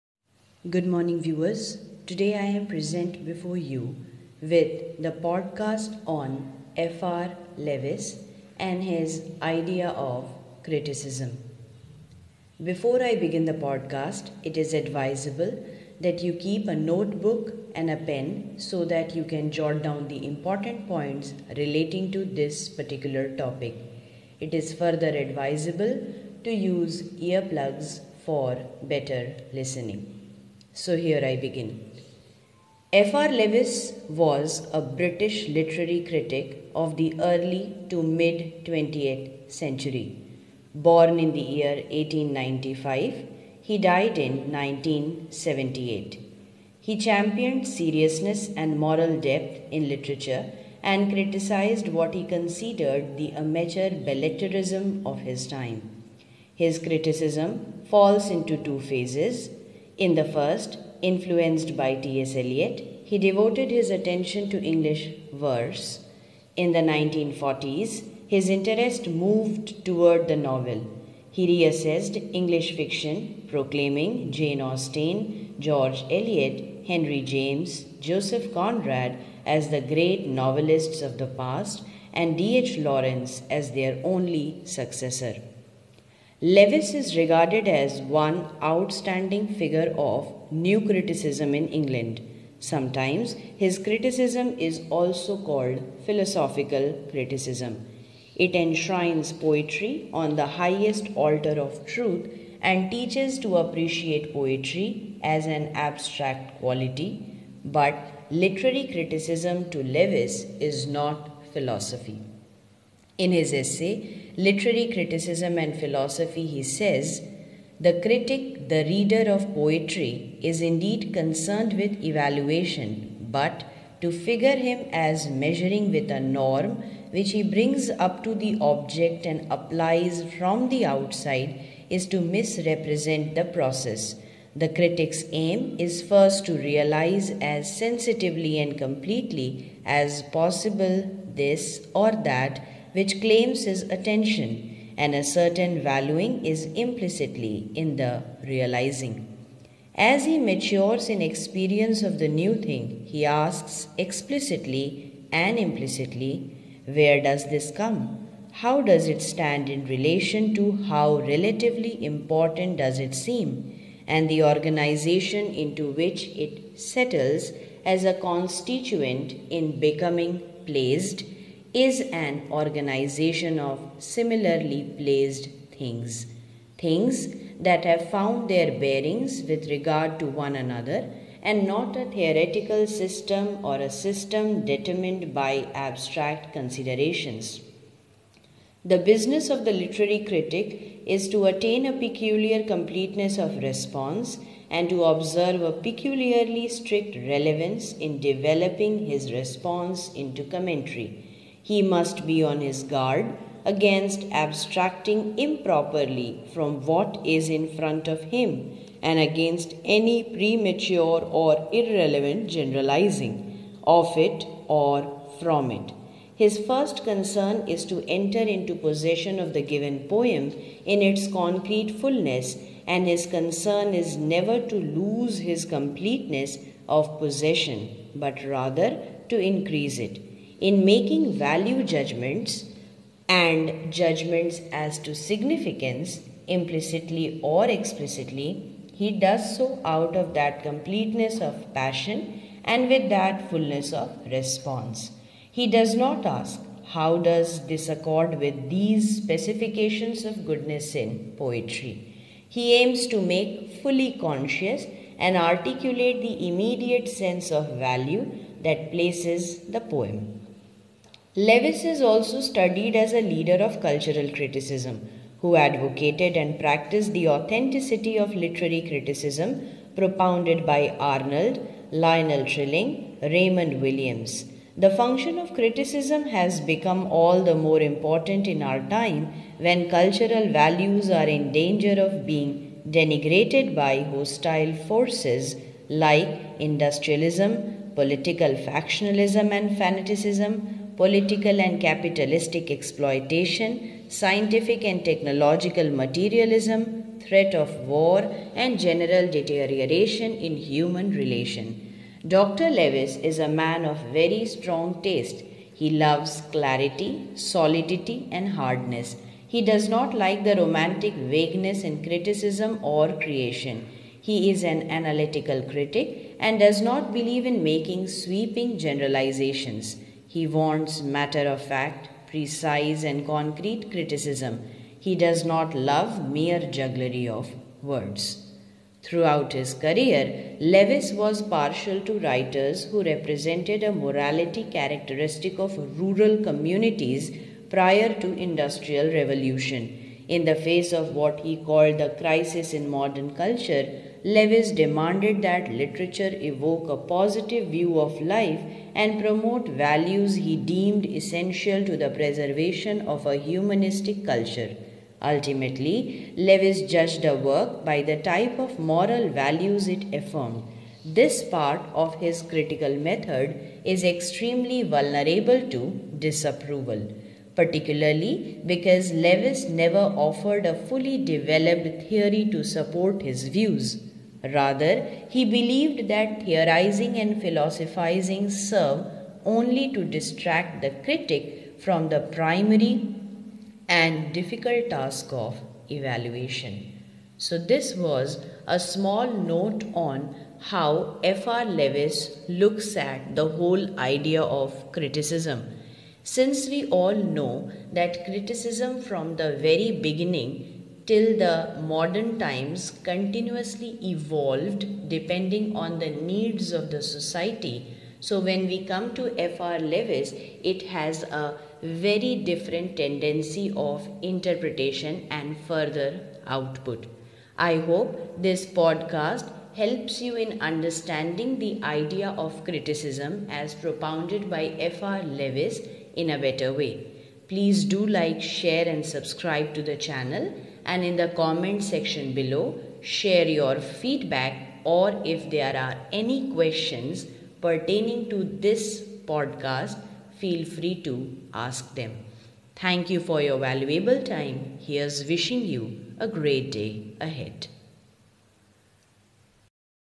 F. R. Leavis and his Criticism | Quarantine Days | Manjari Shukla
Topic : F. R. Leavis and his Criticism

Keep a notebook and pen to jot down the important points relating to the topic
Please use earplugs for listening to the podcast. 😇🙏

Content : Notes from Internet

Video made by : Vlogit 🔆

Keep learning and growing
Love
Manjari Shukla 💕